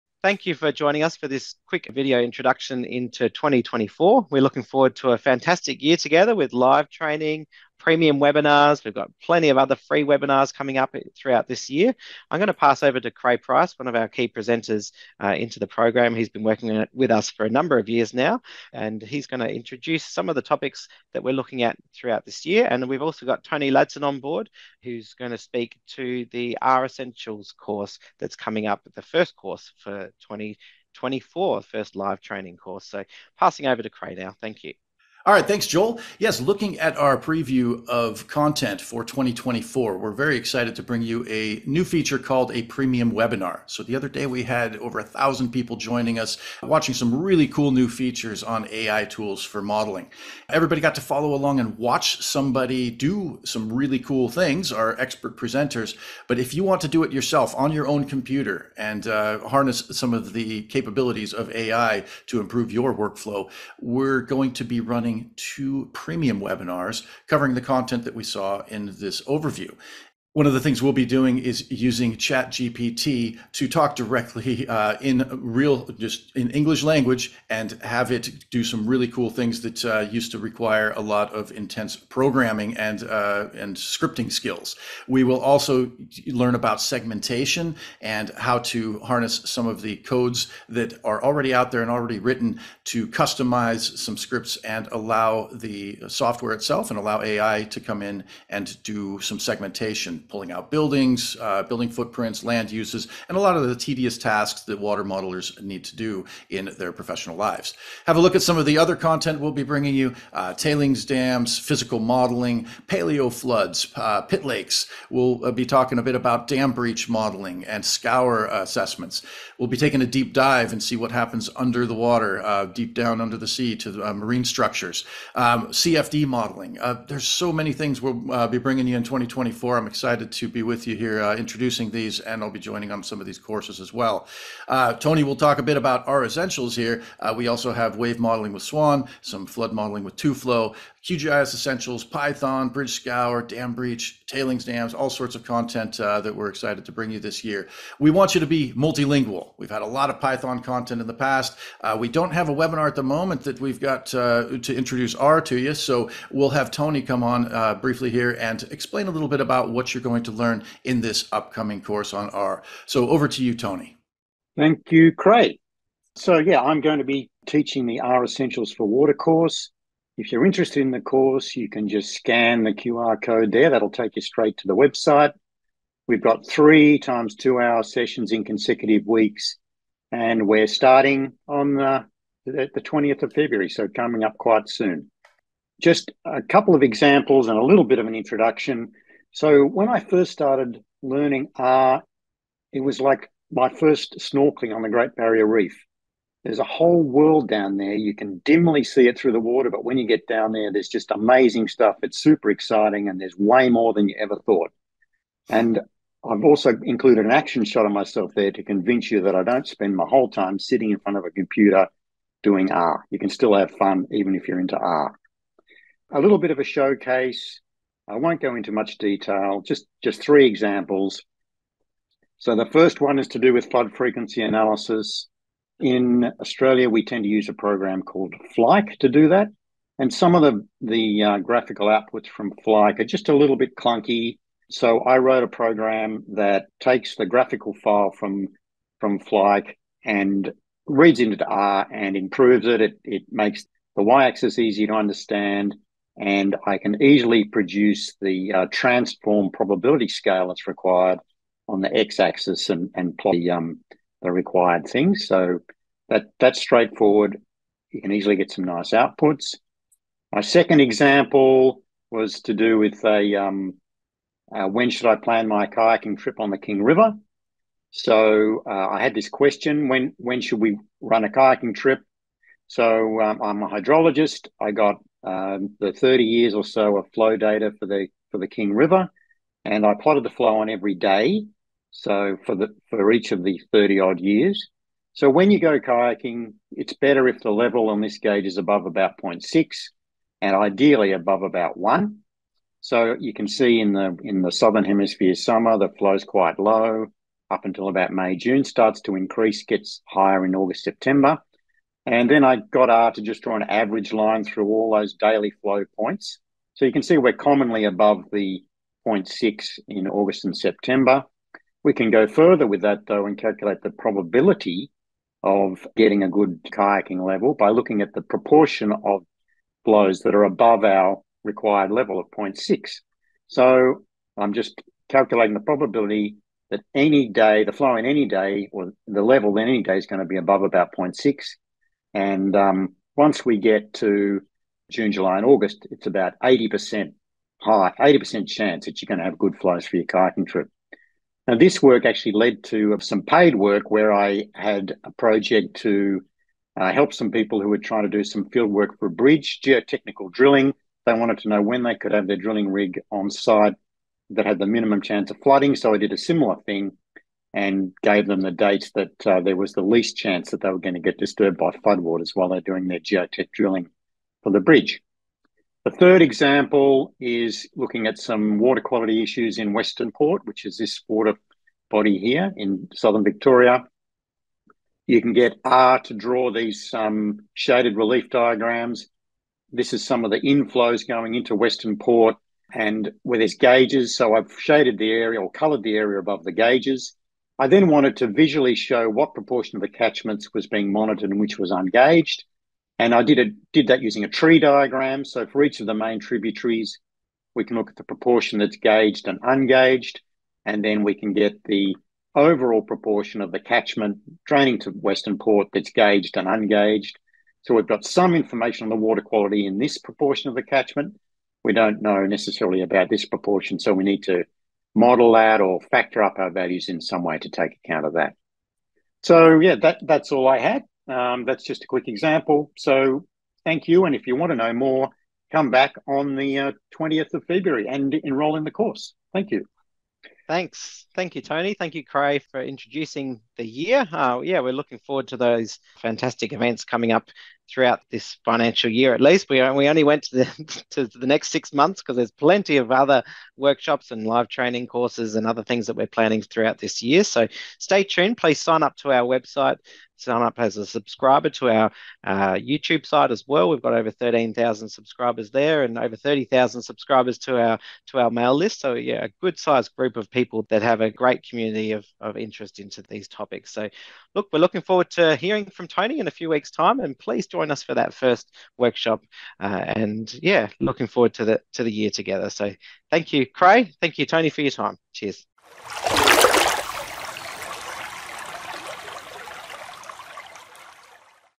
2024 Preview: Dive into AWS Online Training!
Discover the latest offerings from the Australian Water School's calendar of online courses, webinars, and premium webinars for 2024! Join us for an exclusive sneak peek into our upcoming sessions, featuring renowned presenter Krey Price. Explore how AI can revolutionize and optimize your hydrological models and outputs, with a plethora of webinars and brand-new premium offerings.

Plus, delve into the world of hydrology with expert Tony Ladson, boasting three decades of experience in the field. Learn about our upcoming R essentials online course, commencing on February 20th. Tailored for beginners, this live training experience empowers water professionals with fundamental skills to leverage R for hydrological analysis. From data loading to model fitting, participants will gain hands-on experience in data handling, visualization, and scripting directly applicable to hydrological datasets.

Uncover the power of R in visualizing trends, patterns, and relationships within your data, essential for informed decision-making in water management and conservation efforts. Don't miss out on this opportunity to streamline your data processing workflows and enhance your skillset with our engaging and practical lessons. Join us and embark on a journey towards mastering R for hydrological excellence!